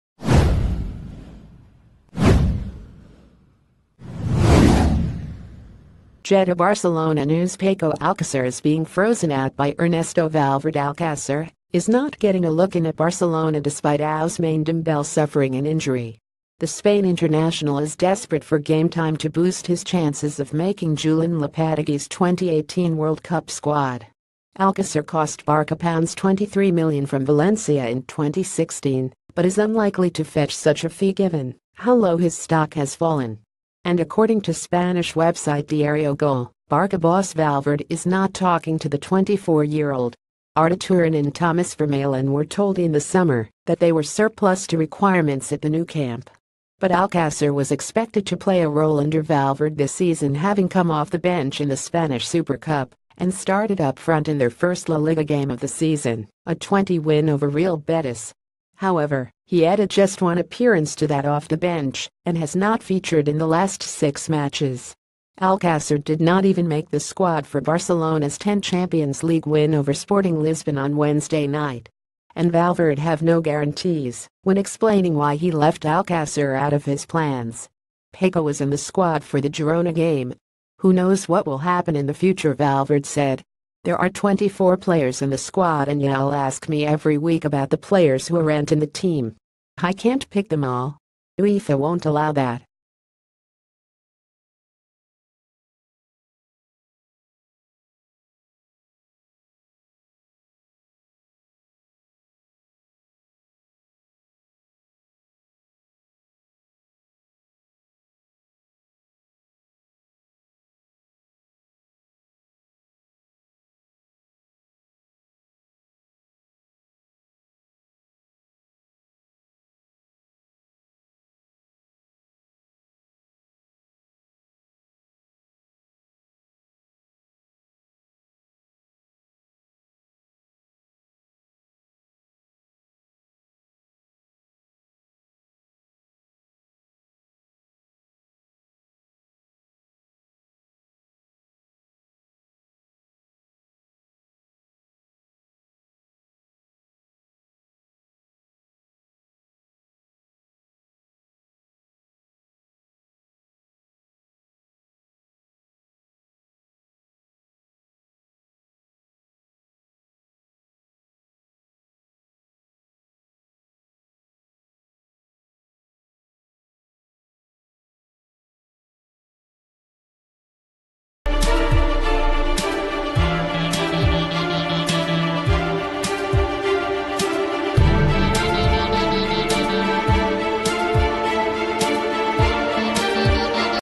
Breaking News | Barcelona news: ernesto valverde is not talking to one barcelona player
Barcelona news: ernesto valverde is not talking to one barcelona player

GETTYBarcelona news Paco Alcacer is being frozen out by Ernesto ValverdeAlcacer is not getting a loo...